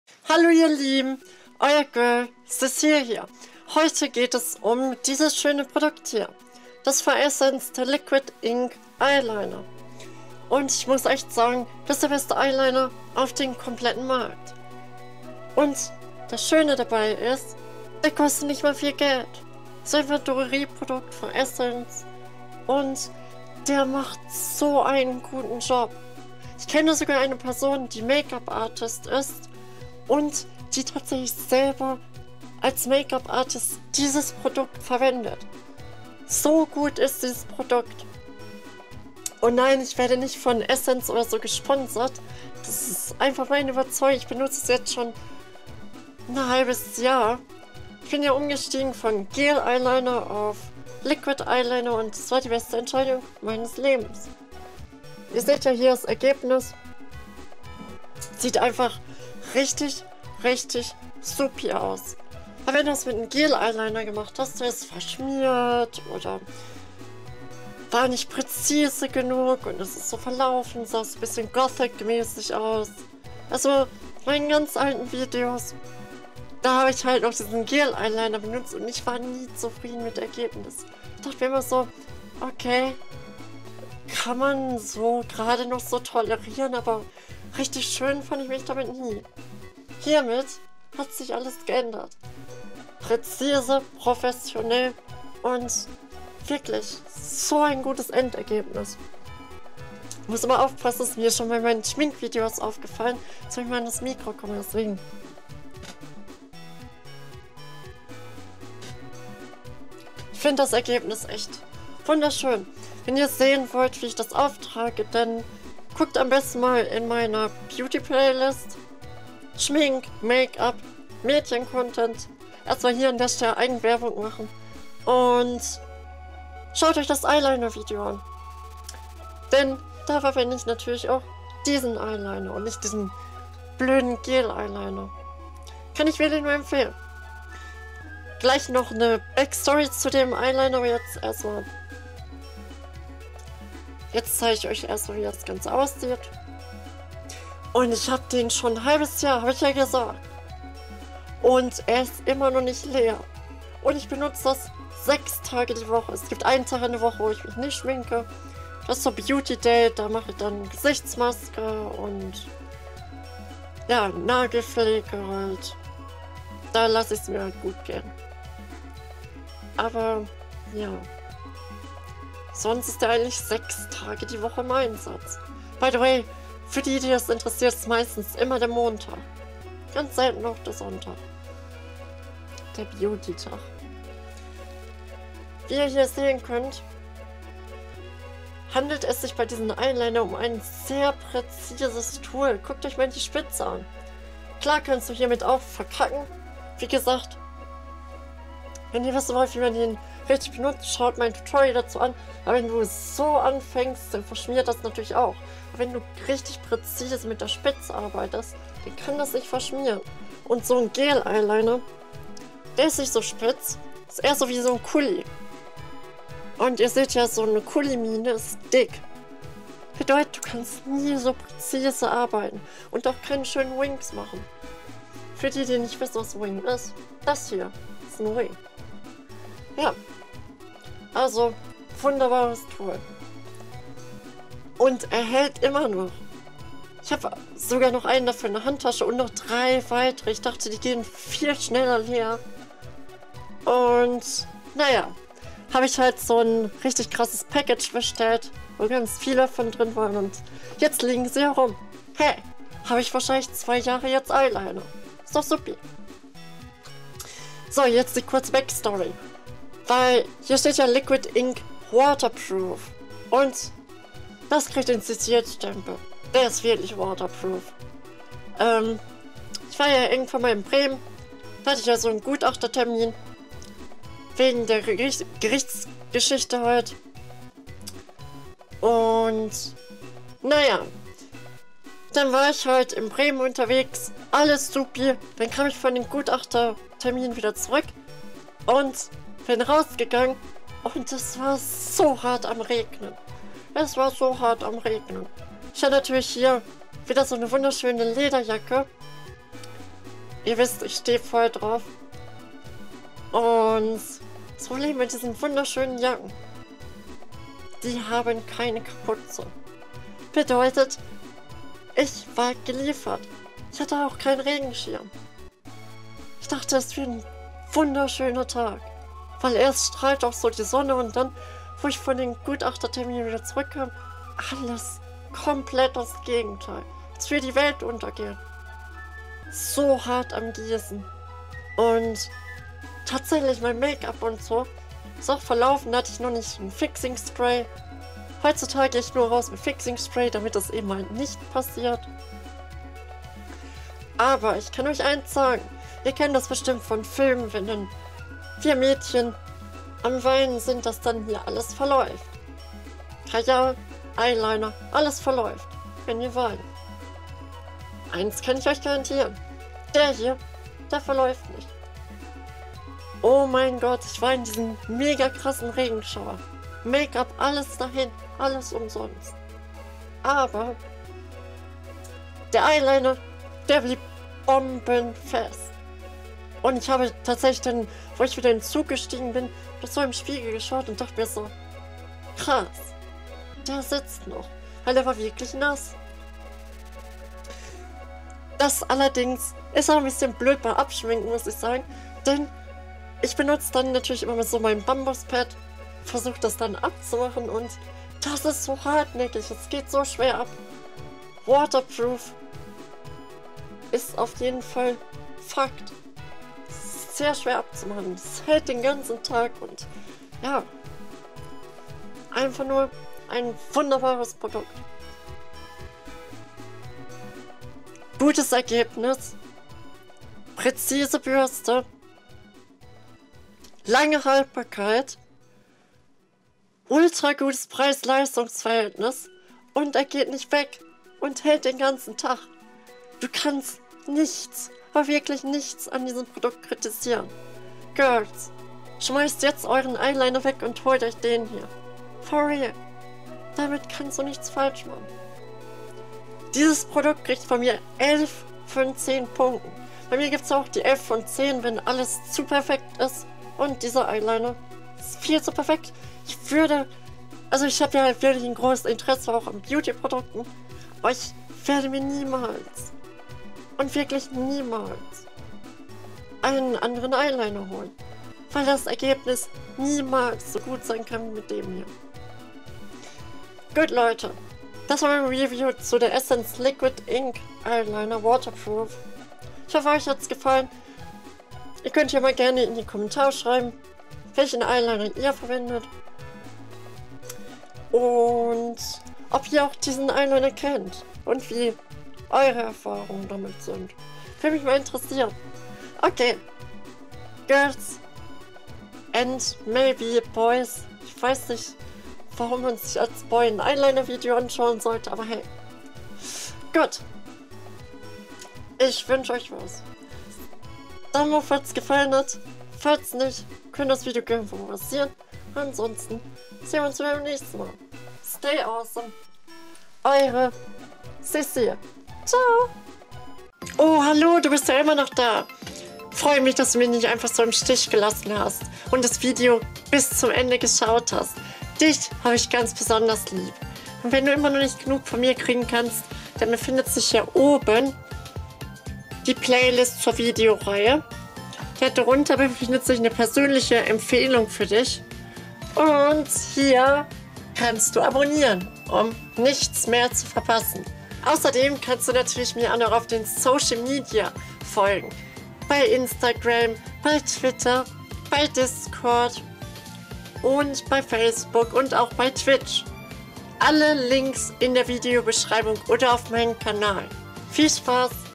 REVIEW: Essence Liquid Ink Eyeliner Waterproof [German/4K] Der beste Eyeliner! - Cécile Akira 👸🏼 ❤
►Du willst mir Geld spenden?

‎( ͡° ͜ʖ ͡°) HELP ME PLEASE!!! ‎( ͡° ͜ʖ ͡°)

Ansonsten wünsche ich Euch noch einen schönen Tag, bis dann und ciao und danke für's Anschauen! Haut rein! :D